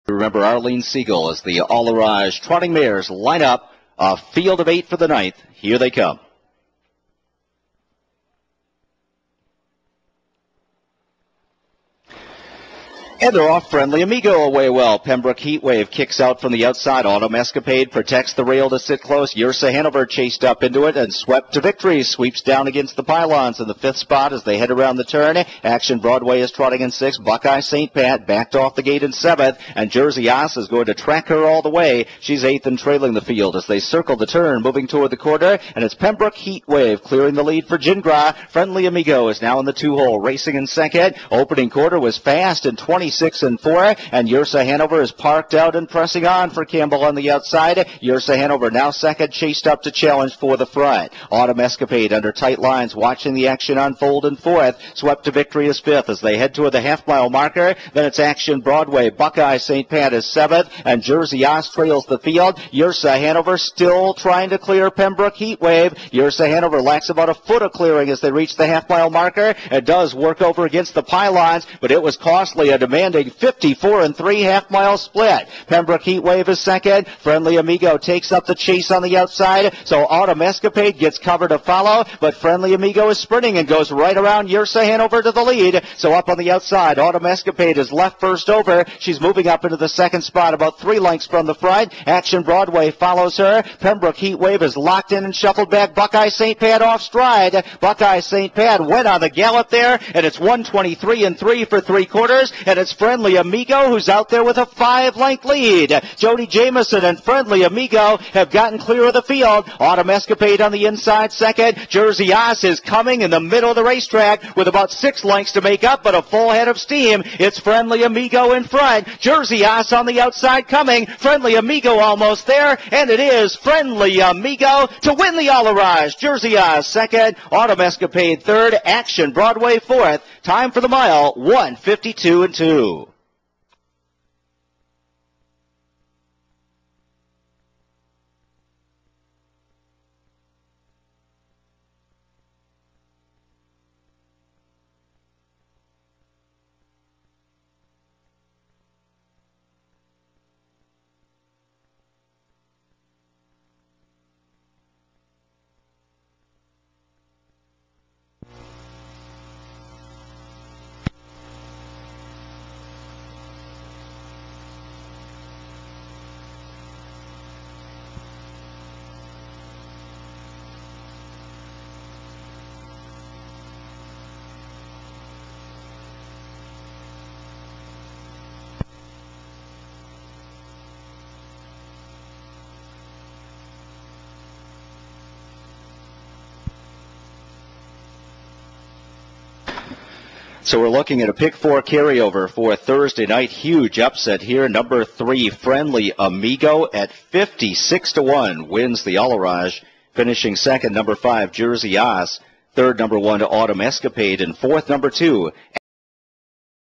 Allerage Farms Filly Trot 2011 -Friendly Amigo
Lexington-Usa, USD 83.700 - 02/10/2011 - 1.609mt
1 Friendly Amigo, J. Jamieson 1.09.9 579
(Amigo Hall - Diminutive)
2 Jersey As, G. Brennan 1.10.1
3 Autumn Escapade, D. Miller 1.10.4
4 Action Broadway, B. Sears 1.10.5
5 Yursa Hanover, J. Campbell 1.10.9
6 Pembroke Heat Wave, Y. Gingras 1.11.5
7 Swept To Victory, J. Juel 1.11.6
8 Buck I St Pat, T. Tetrick rp